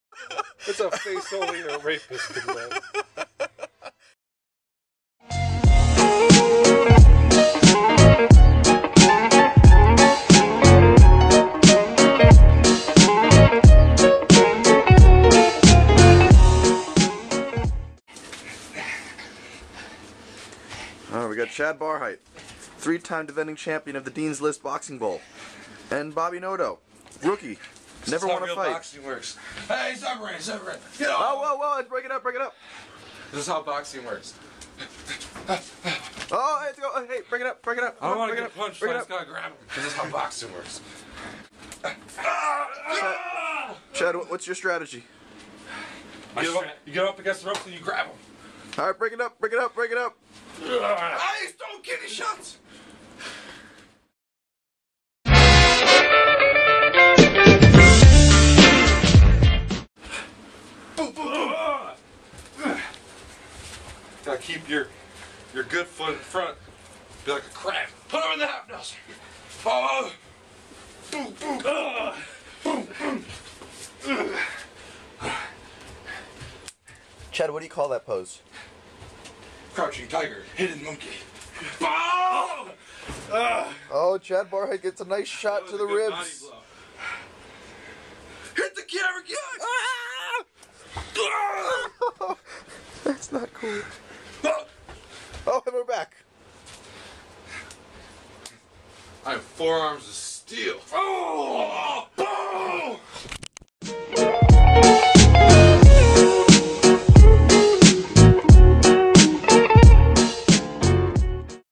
CA Productions: Title Fight
Chad the three time defending champion of the Deans List Boxing Championship takes on Bobby the newcomer.